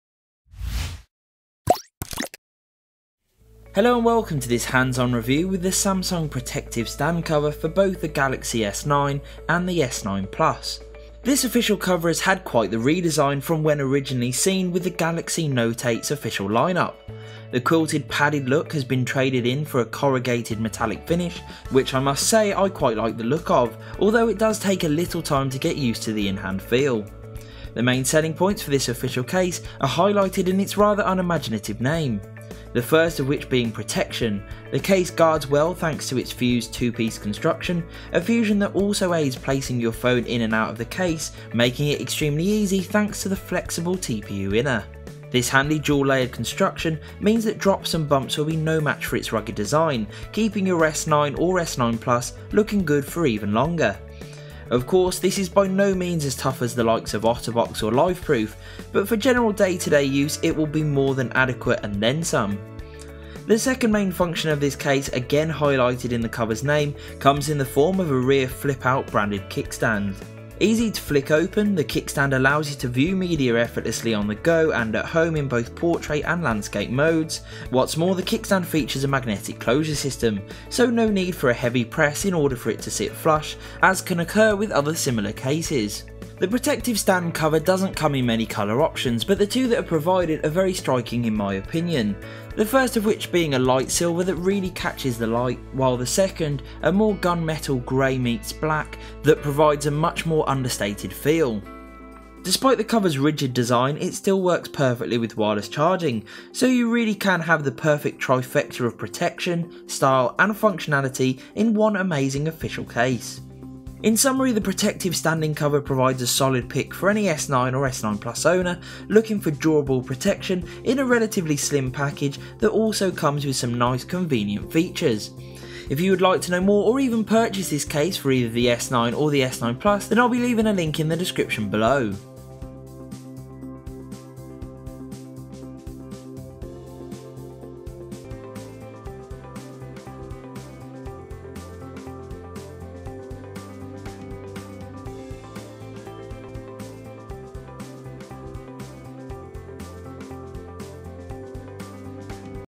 Official Samsung Galaxy S9 / S9 Plus Protective Stand Cover Review
In today's video we'll be reviewing the Official Samsung Protective Stand Cover for the Galaxy S9 and the S9 Plus. Providing superb protection and top functionality, this official cover represents a top choice for any S9 owner looking to add solid protection, with amazing style.

Social Links...